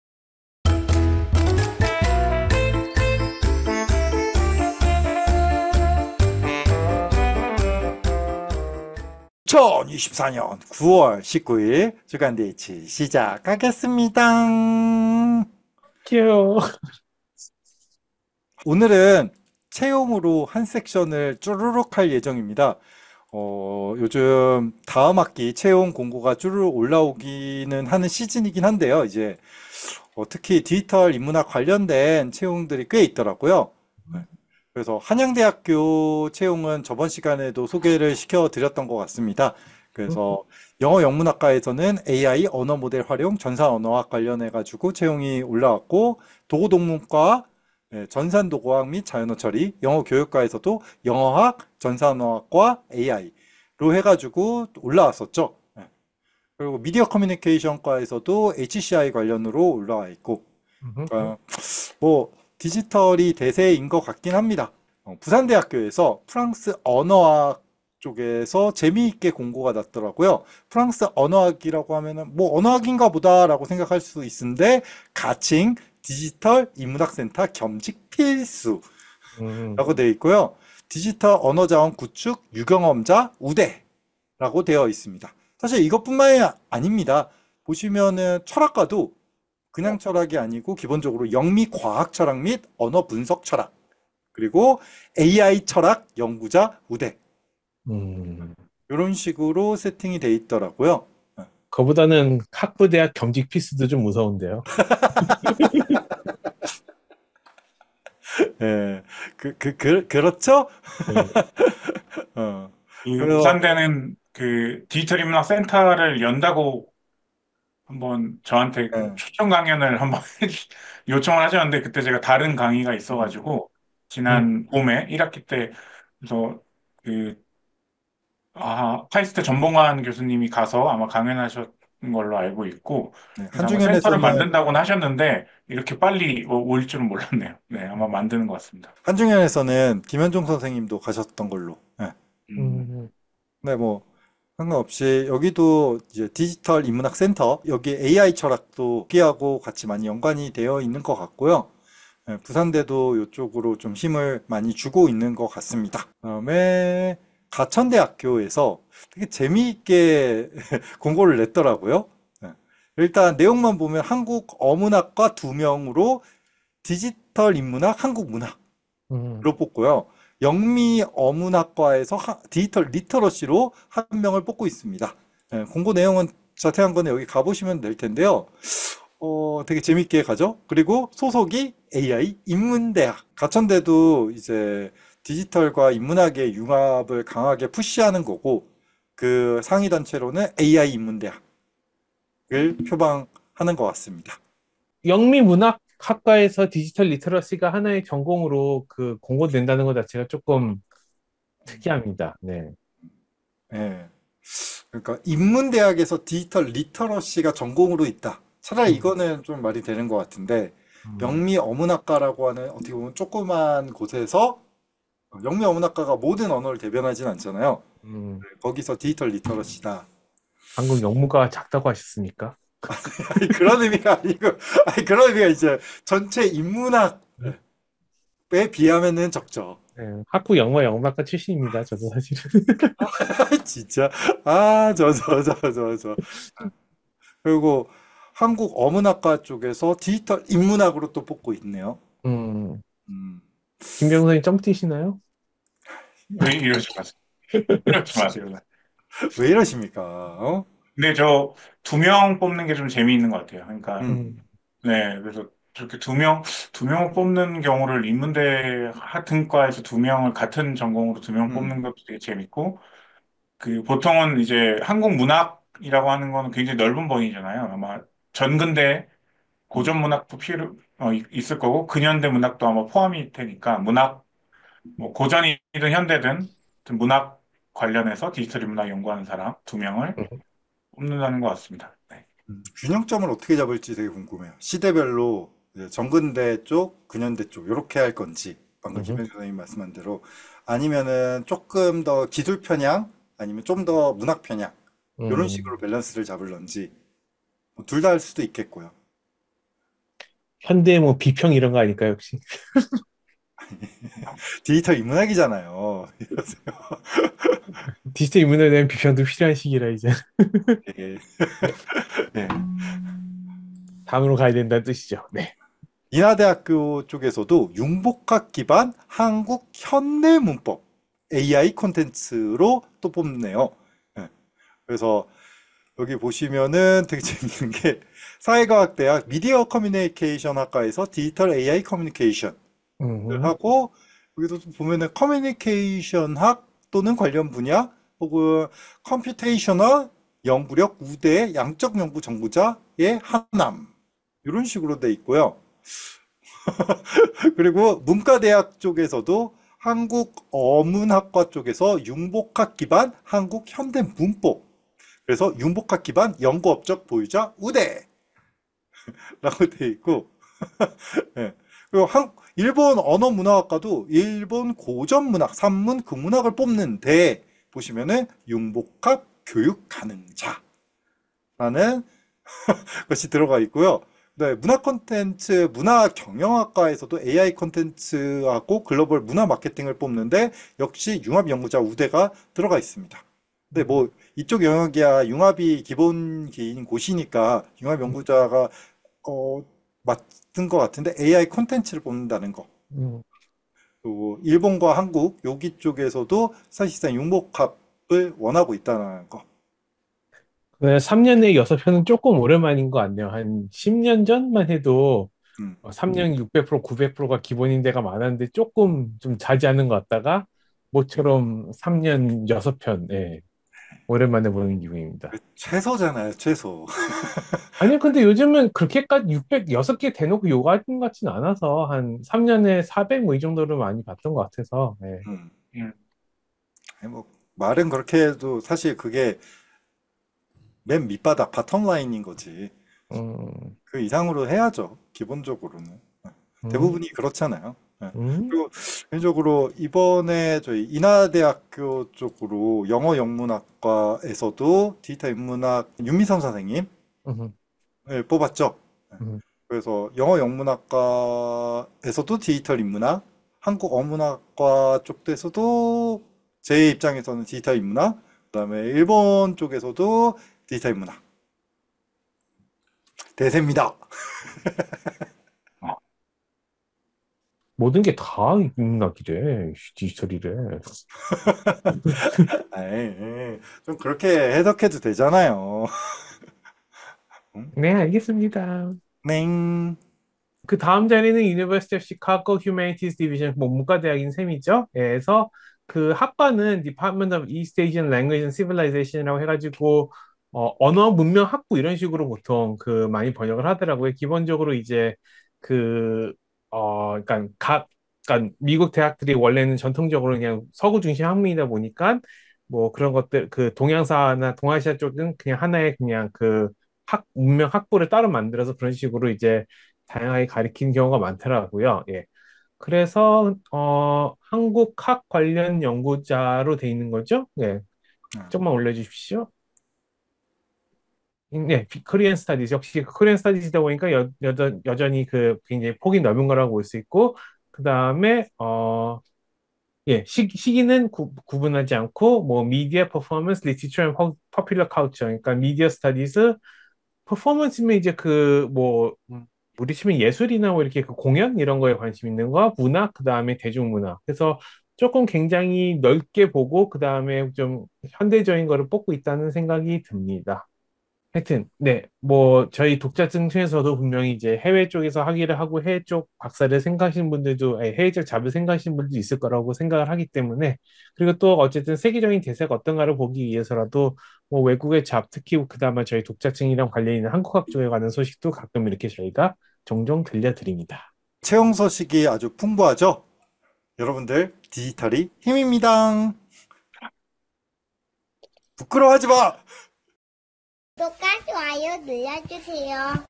[주간DH] 전임 / 한양대+부산대+가천대+인하대+시카고대
[채용] 전임 / 한양대 2025학년도 1학기

[채용] 전임 / 프랑스언어학(디지털 인문학센터 겸직) / 부산대 2025학년도 1학기

[채용] 전임 / 디지털인문학, 디지털 리터러시 / 가천대 2025학년도 1학기

[채용] 전임 / 융복합 기반 한국현대문법, AI 콘텐츠 / 인하대 2025학년도 1학기

[채용] 전임/ The University of Chicago: Humanities Division: Department of East Asian Languages & Civilizations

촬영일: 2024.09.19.
참가자: 김병준, 조원희, 김바로

인트로 제작: 전대리(였는데 지금은 이제 팀장이 된 B급 영상 마스터)
아웃트로 제작: 전가람(편집), 구현회(그림), 김마루(목소리)

* 본 영상은 발편집을 지향합니다.